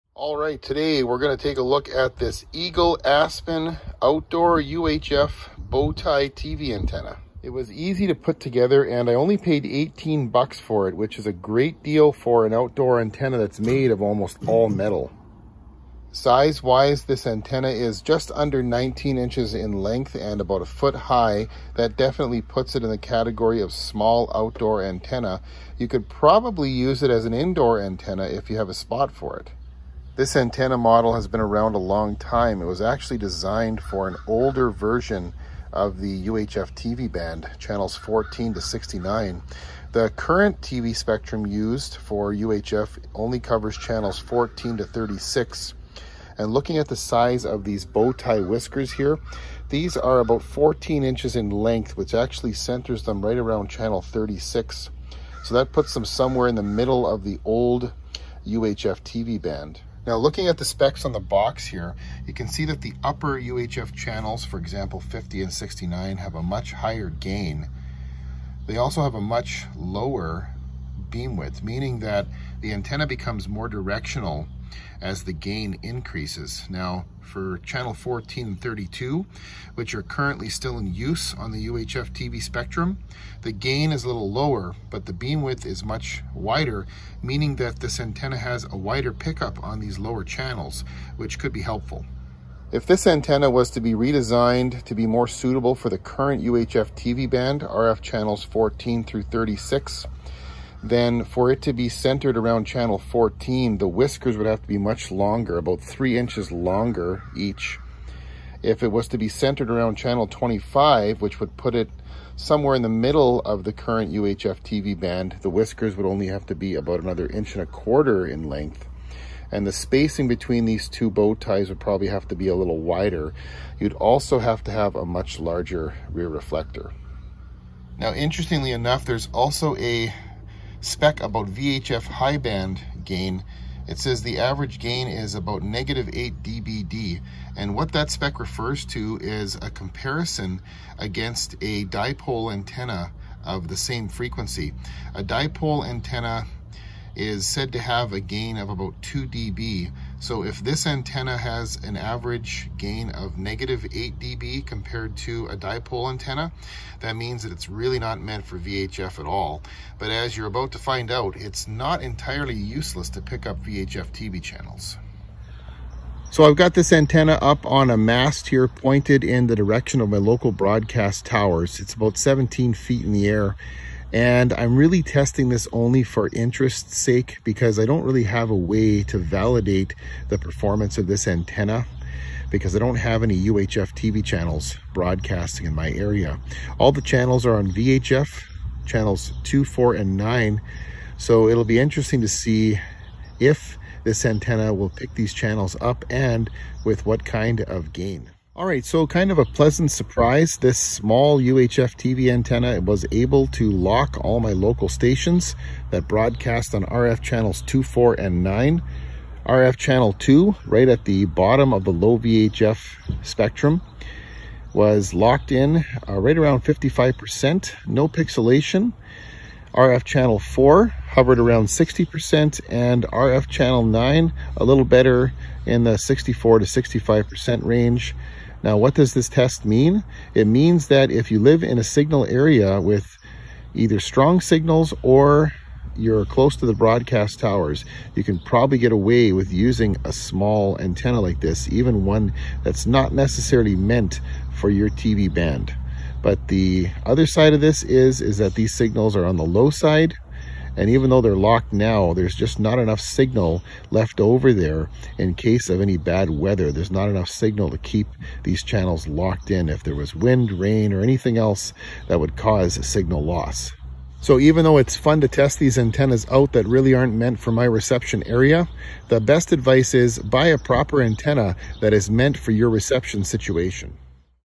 Eagle Aspen UHF Bow Tie TV Antenna - OTA TV
Eagle Aspen UHF Bow-Tie HDTV Antenna for OTA over the air television. Model DTV2BUHF. A bowtie TV antenna that has been around a long time. I bought it from Solid Signal.

Turning old Rabbit Ears into an outdoor TV Antenna for Free Over the Air TV Channels